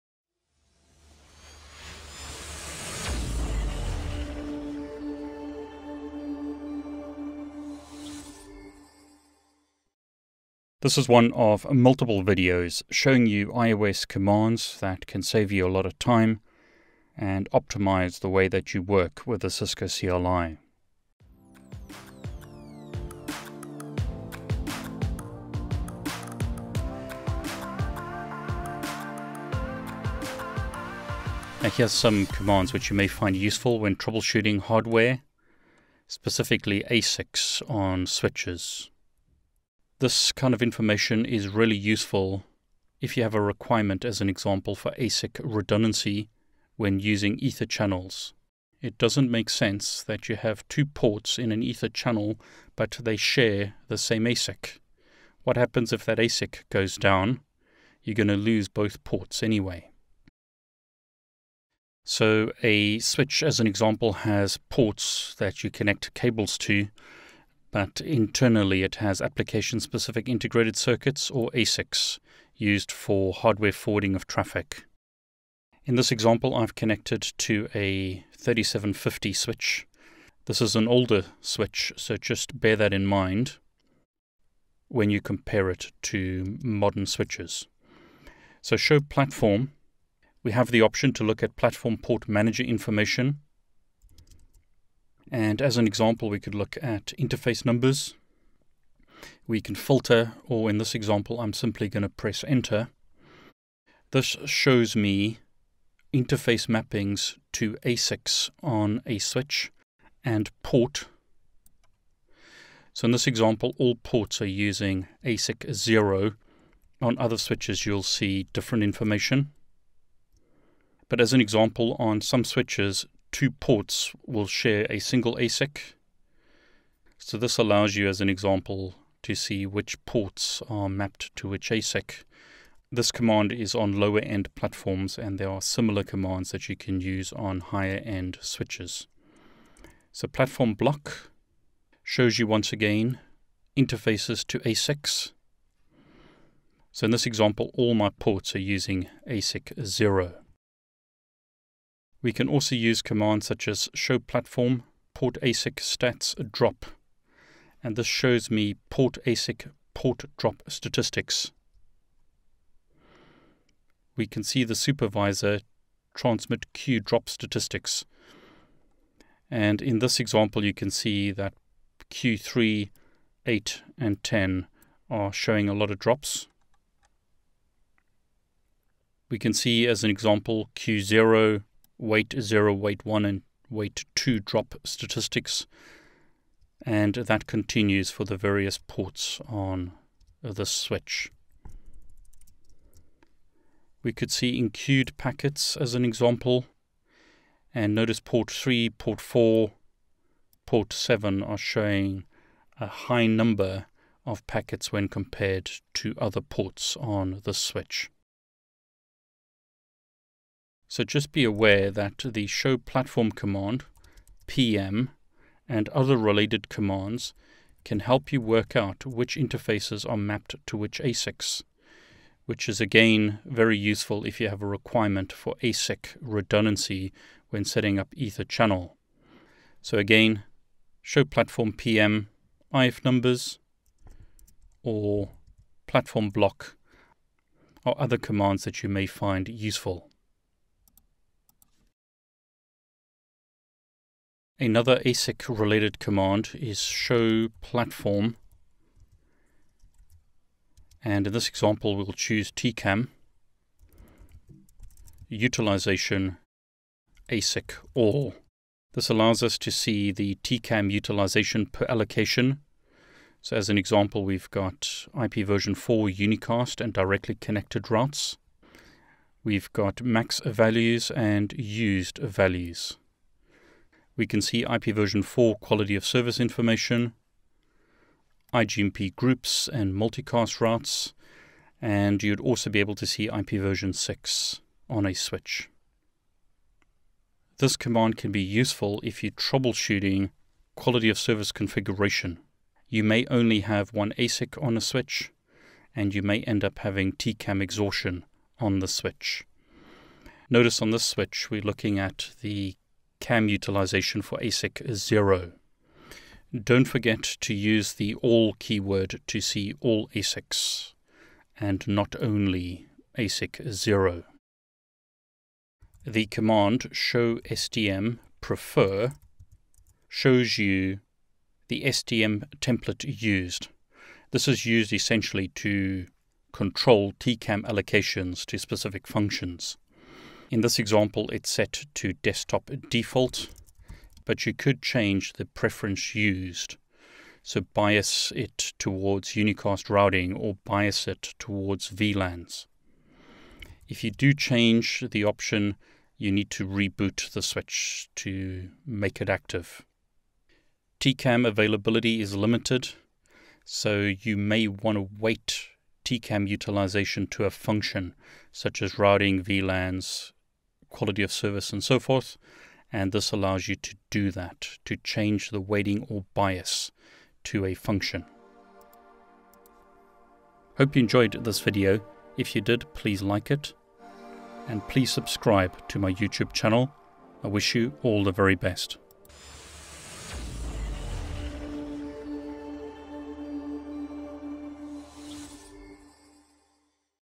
GNS3 Talks: Cisco IOS time saving commands and tips (Part 9)
Save time when working with the CLI by using cool commands and tricks. This is one of multiple GNS3 time saving commands videos.

Also includes content for the ICND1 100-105 and ICND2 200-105 exams.

What do you want to learn? Let me know via the comments.